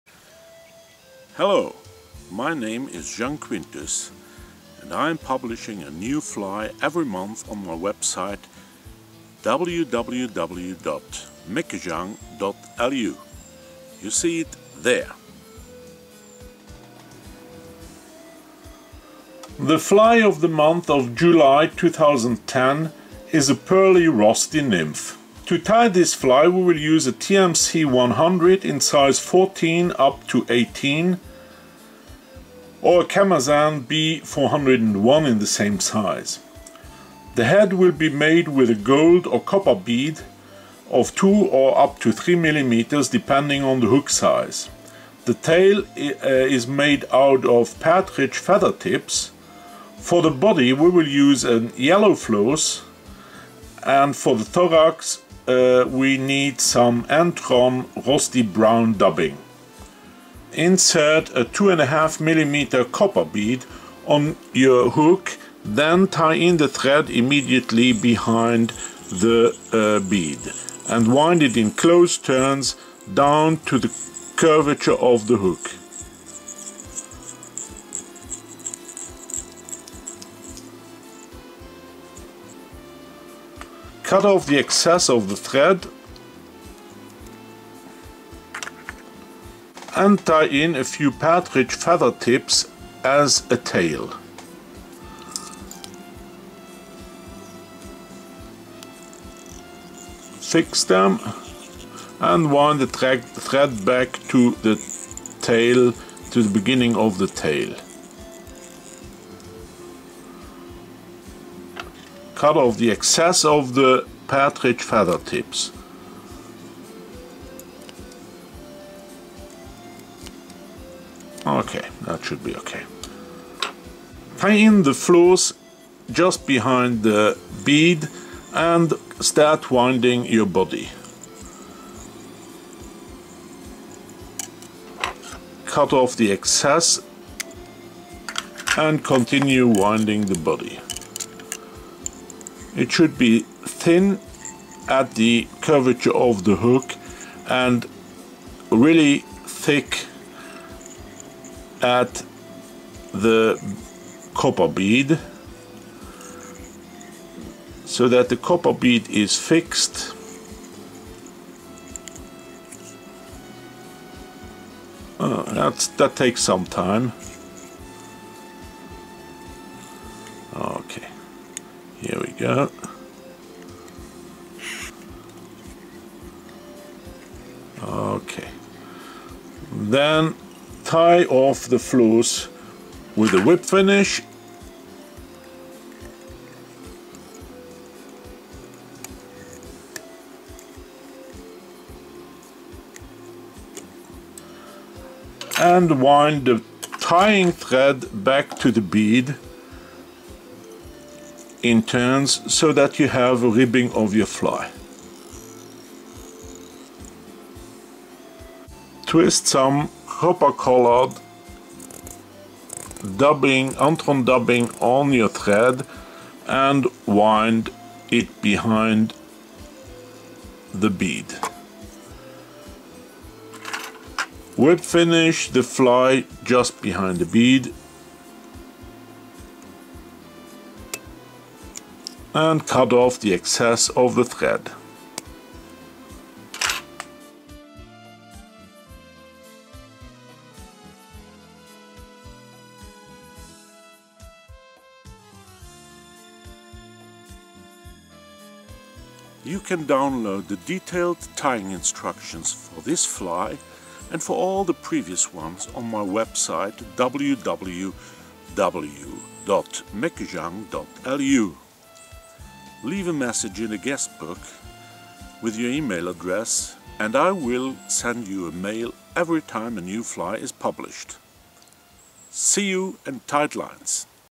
Meckejang Pearly rosty nymph.mp4
Fly of the month of July 2010  tying instruction. The pearly rosty nymph  fly from Luxembourg